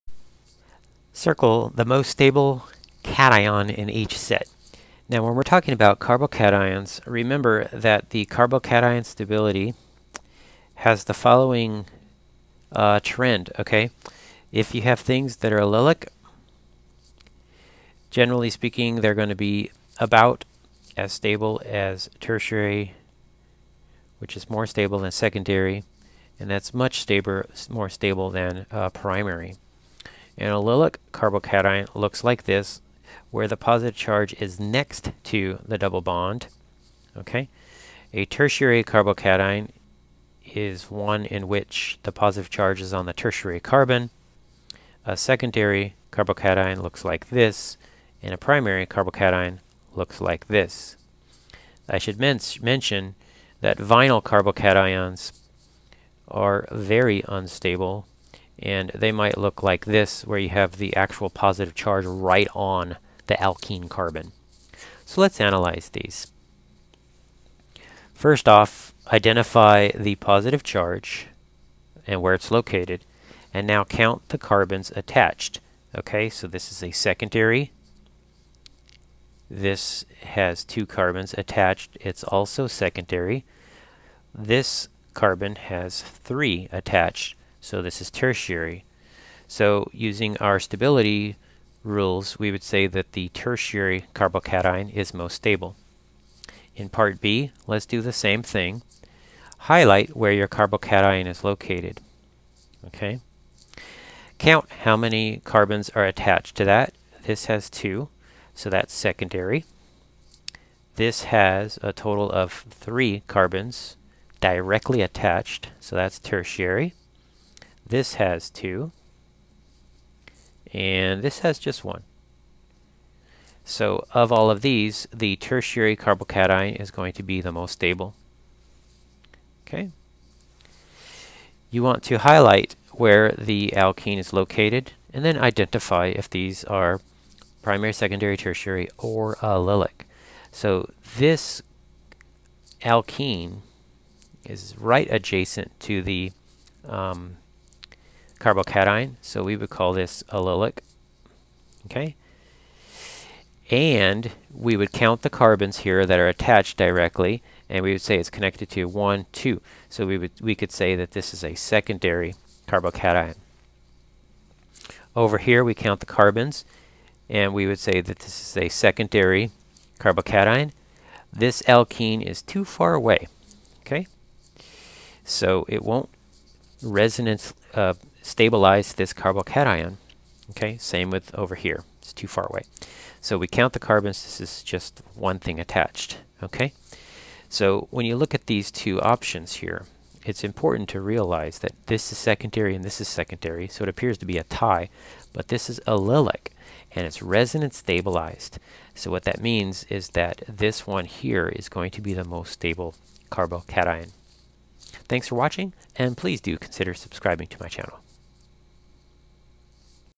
carbocation stability review
This tutorial helps you identify and determine which carbocations are most stable.